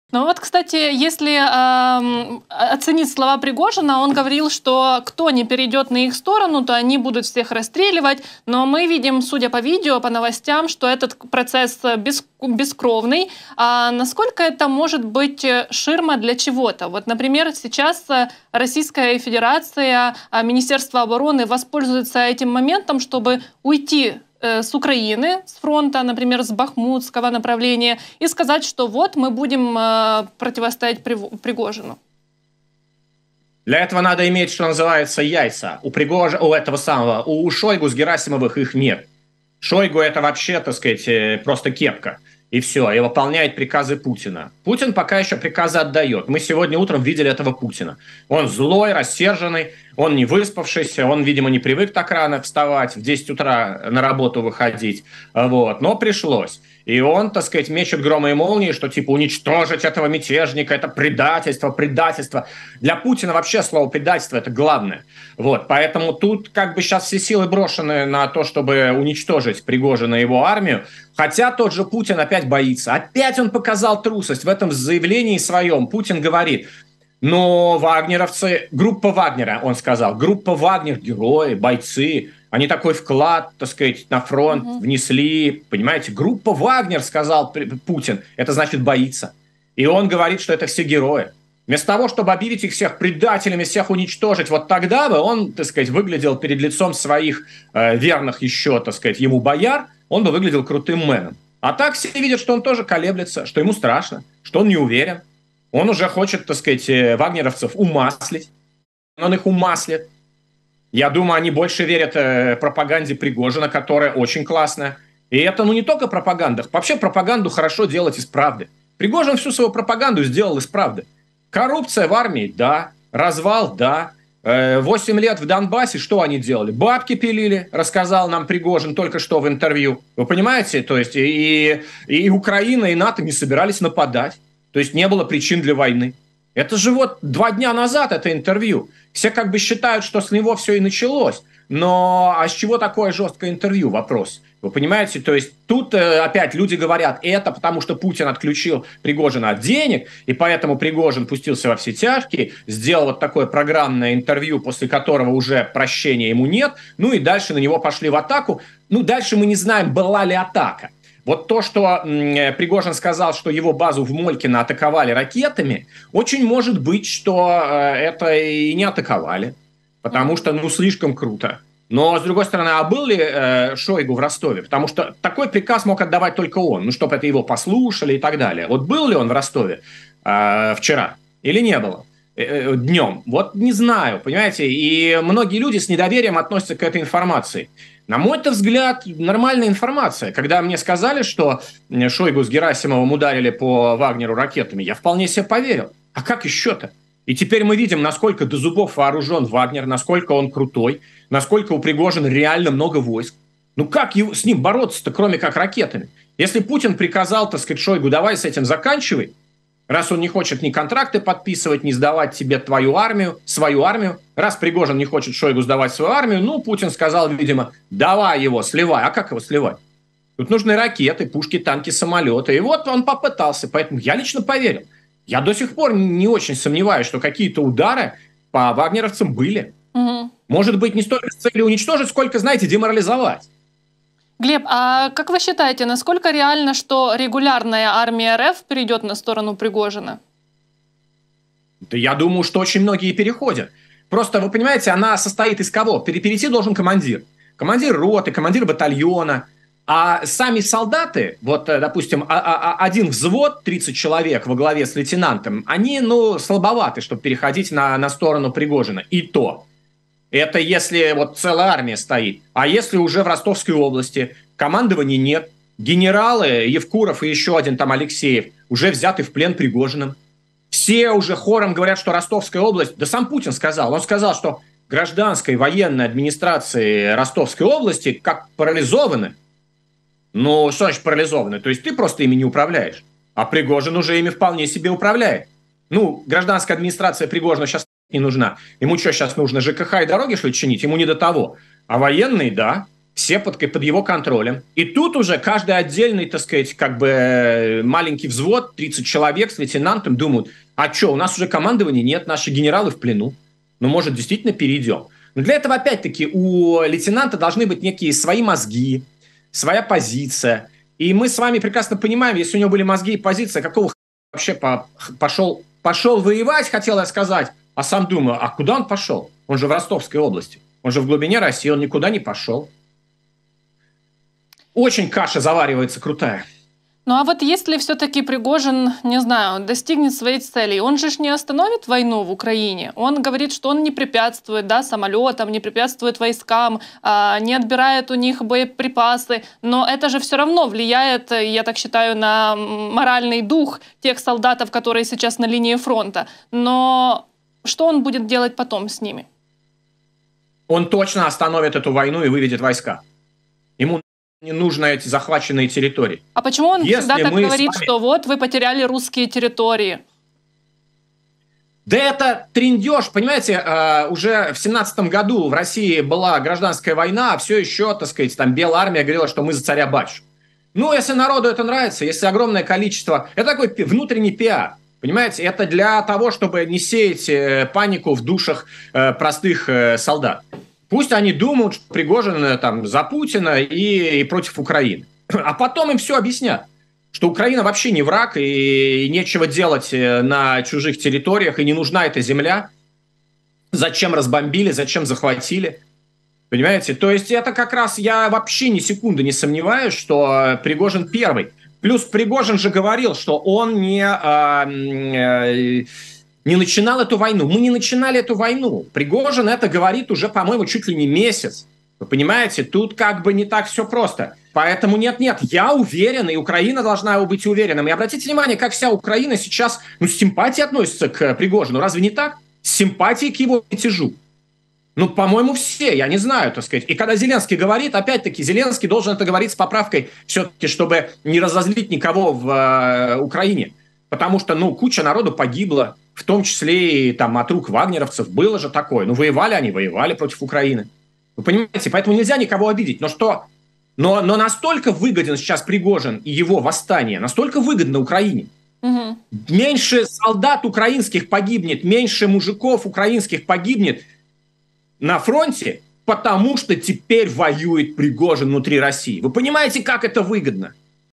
П’ЯНИХ: "СВО" закінчилась? / Пригожин може змусити Путіна ВІДВЕСТИ війська з України
Російський опозиційний журналіст та відеоблогер Гліб П'яних @glebpiano  прокоментував державний переворот у РФ. Крім того, він припустив, чи виведуть війська Росії з Україною.